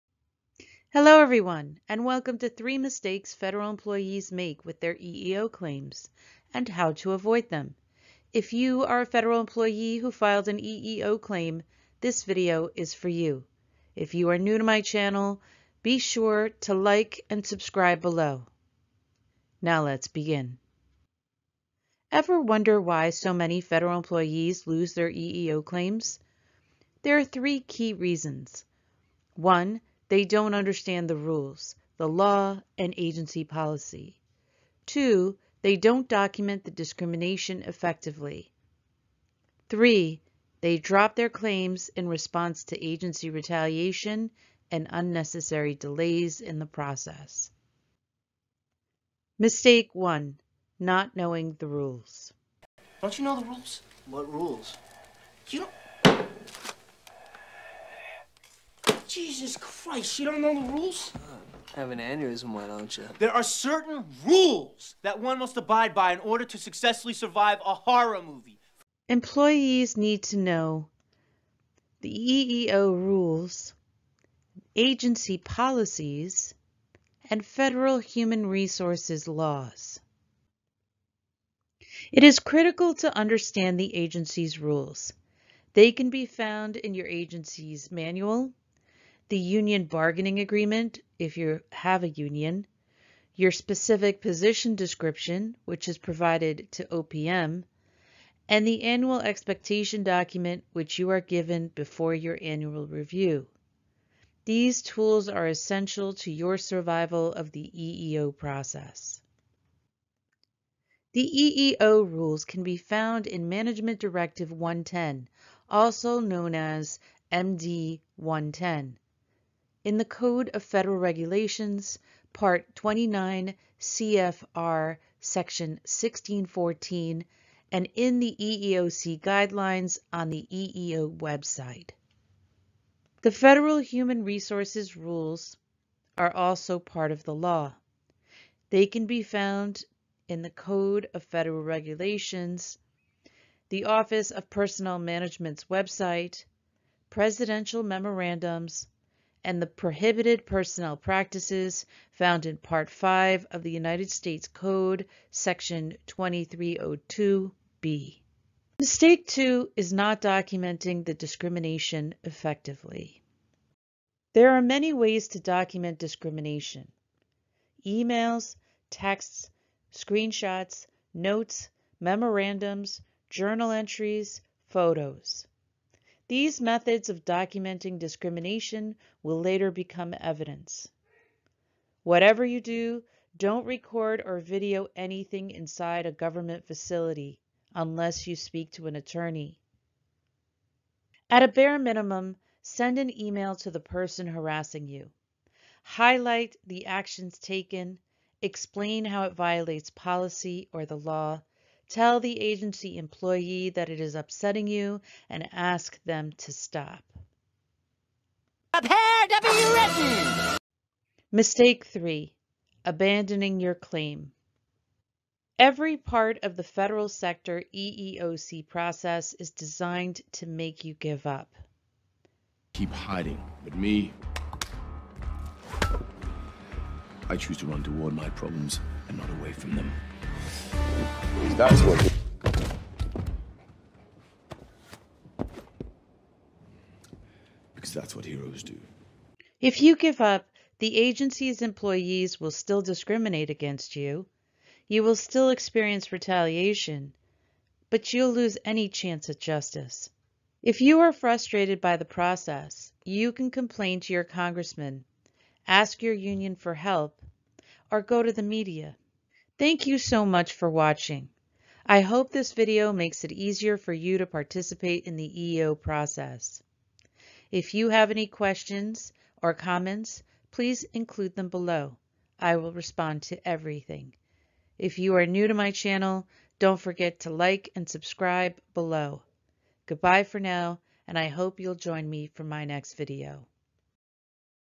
EEO Claims for Federal Employees: Avoiding Common Mistakes - An Attorney's Guide
Filing an Equal Employment Opportunity (EEO) claim can be a complex process, and it's easy to make mistakes. This guide, written by an experienced attorney, explains the three most common mistakes federal employees make when filing EEO claims and offers practical tips on how to avoid them. From understanding the complaint process to gathering evidence, this guide provides valuable information and strategies for anyone looking to file an EEO claim. It's an essential resource for any federal employee who has experienced discrimination in the workplace and is seeking to protect their rights and seek justice.

This is general advice. It isn't meant to replace the advice of an attorney who knows your specific circumstance.